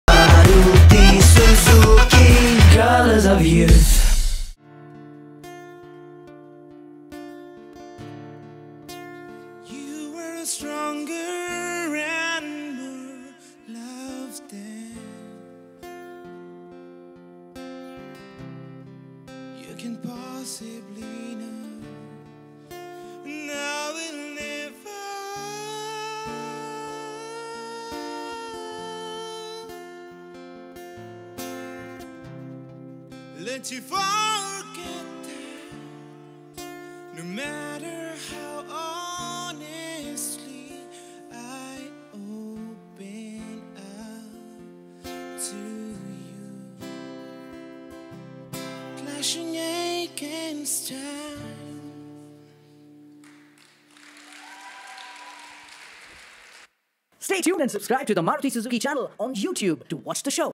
Maruti Suzuki Colors Of Youth Season 8 | Finale | Best Performance | Tanmoy Roy
With his music, he's bringing infinite joy, witness the soothing performance by Tanmoy Roy.